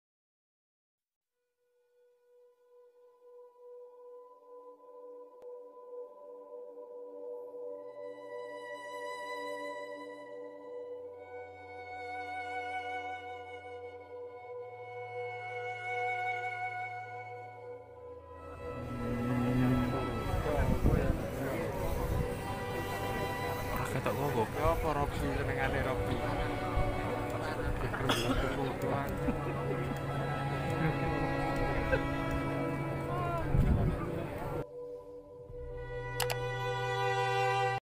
Berita duka tertimbun di sumur#semoga khusnul khotimah .. amiin .
Lokasi : tlogohaji